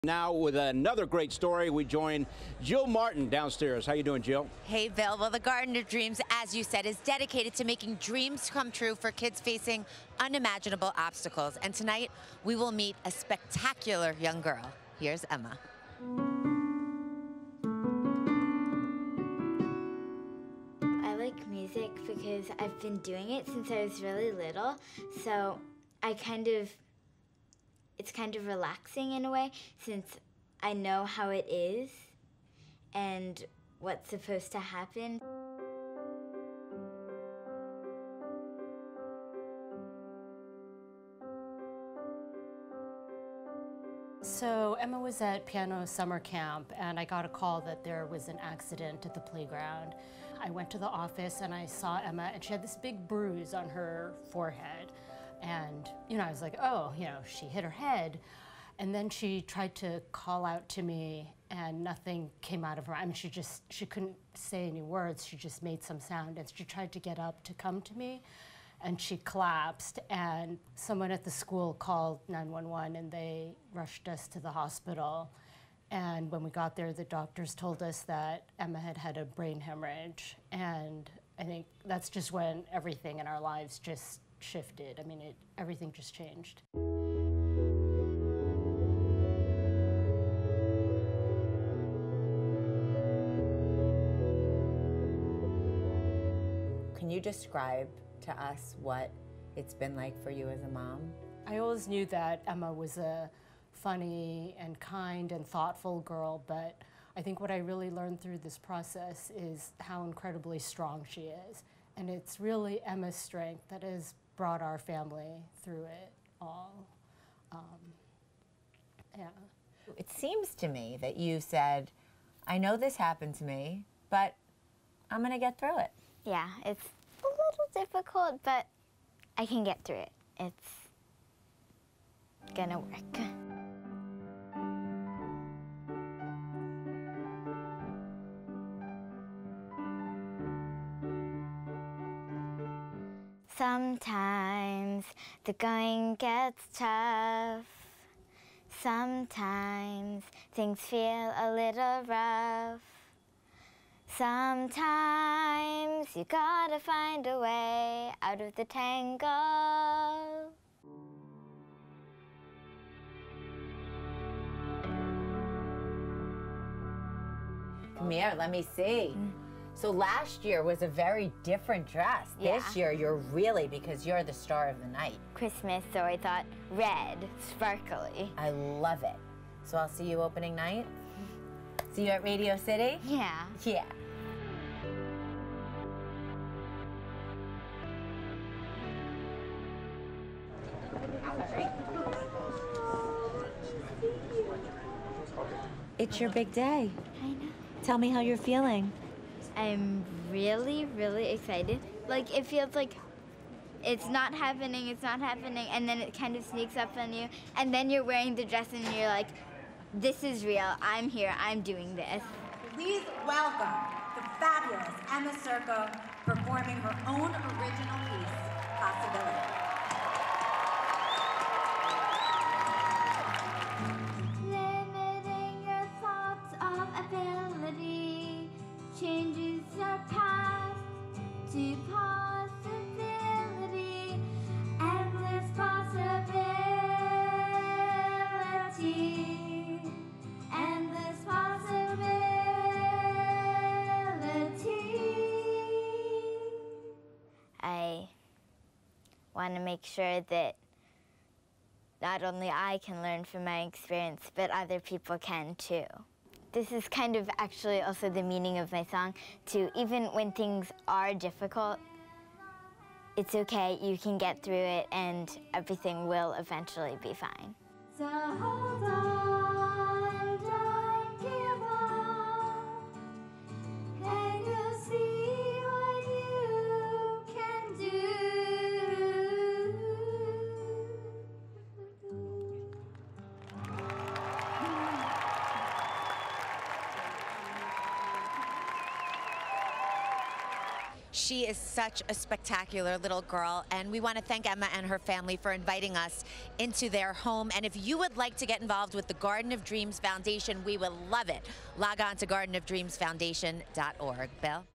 MSG Networks: Emma performs at Christmas Spectacular Opening Night
For more information about the Foundation and to learn how you can support,